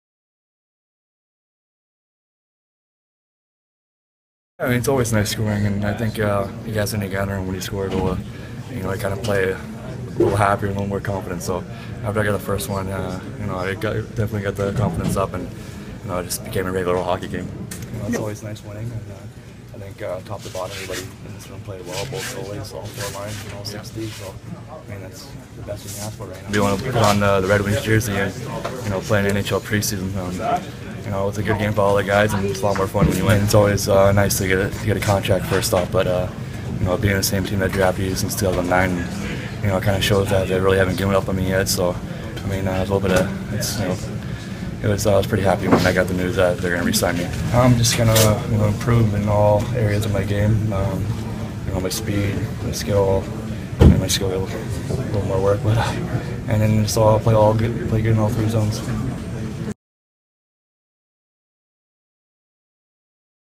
Mitch Callahan scores twice in Red Wings preseason opener
Callahan had the first two goals in a 4-2 win over Pittsburgh.